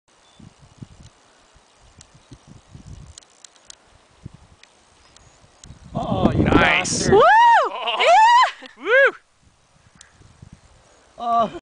Backcountry skiing in Wells
Tyler going over a jump while we were backcountry skiing in Wells Feb 2010